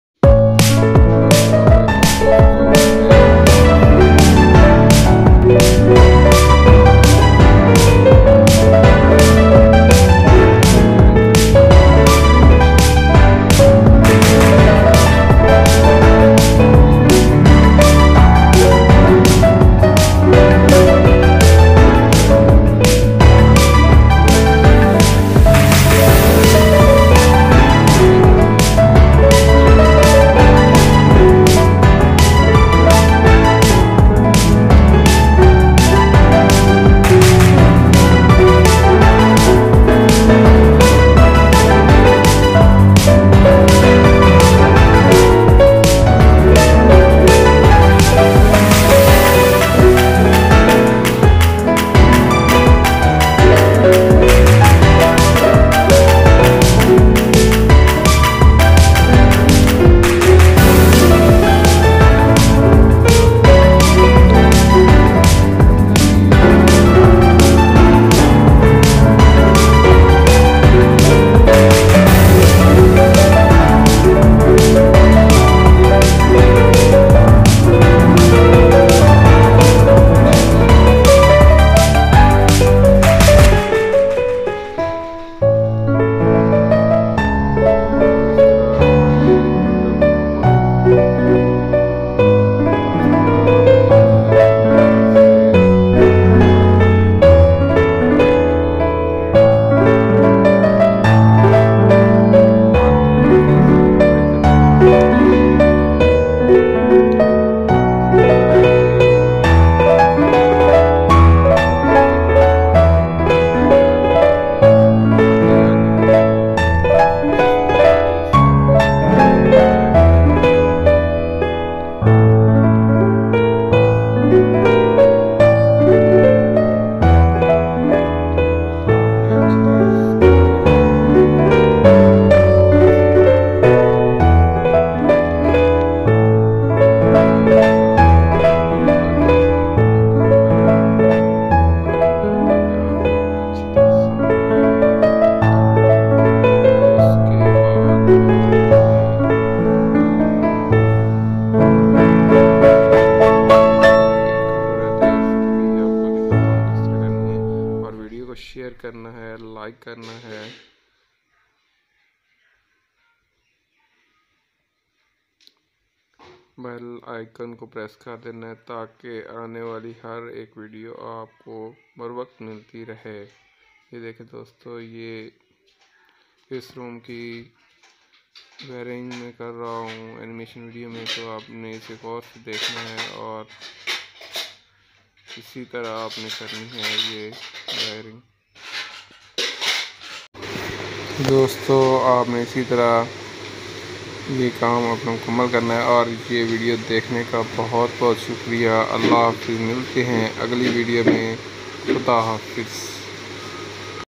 Electrical House Wiring Animation| Ghar Mein Wiring Karne Ka Tarika Animation Video|Sohail#
اس ویڈیو میں بتایا گیا ہے کہ گھر کی وائرنگ کی بنیادی باتیں اُردو اور ہندی میں بتایا گیا ہے
یہ ایک انیمیشن ویڈیو ہے